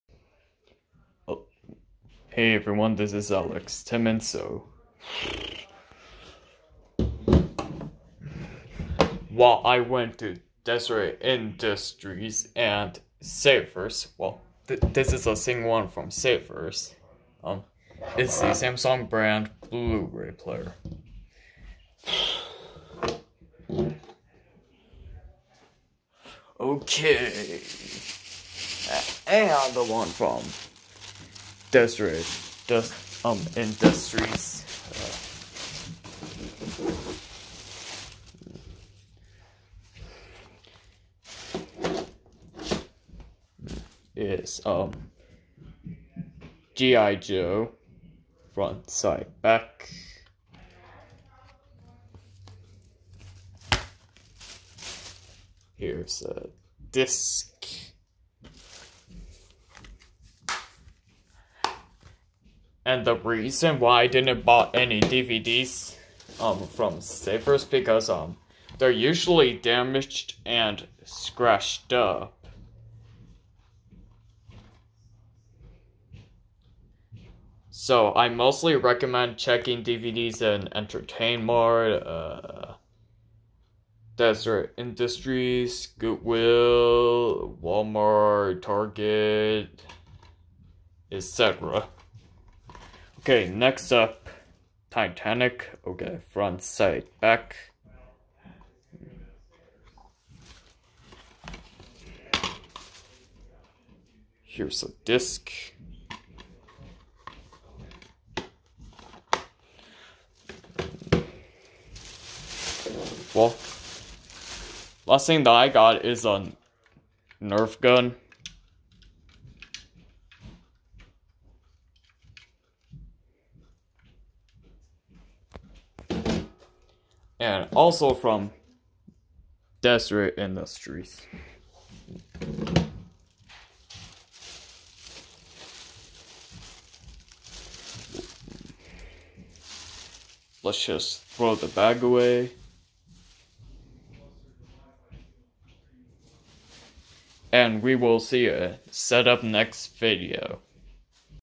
I Finally Bought A Blu-Ray Player From Savers, And Bought 2 DVDs From Deseret Industries
Now I Need An HDMI Monitor